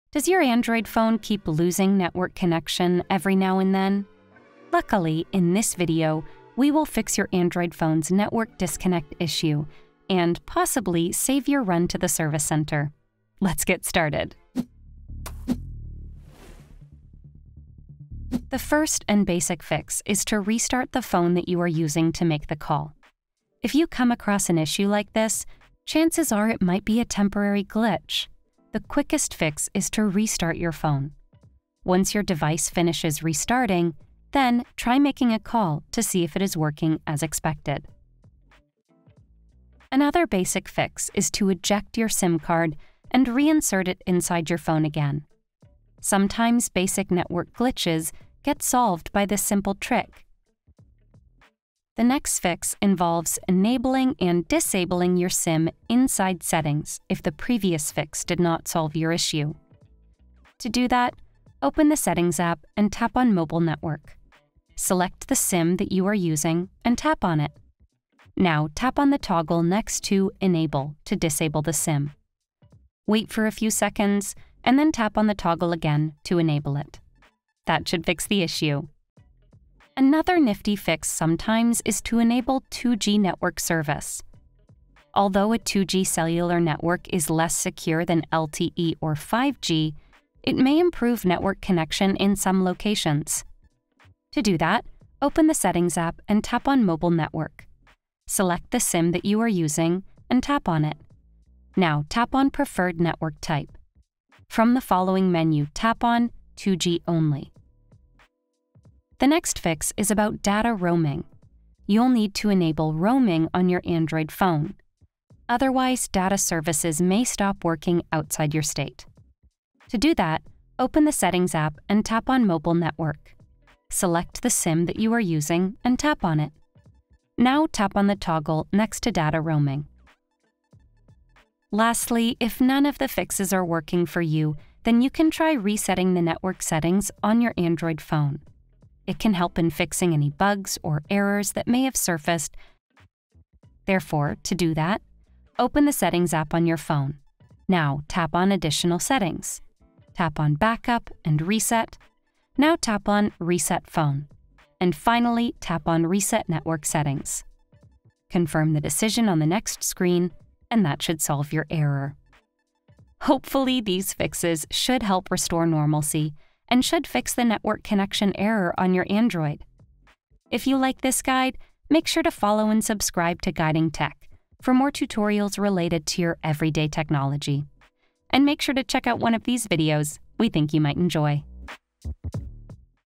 How to Fix Android Phone Keeps Losing Network Connection
Your Android phone is as good as a paperweight if it keeps losing network connection. You can’t make or receive calls and messages or browse the web. There are many reasons for such error.
So, if you are also facing the same issue constantly, then here is how to fix this issue in this video.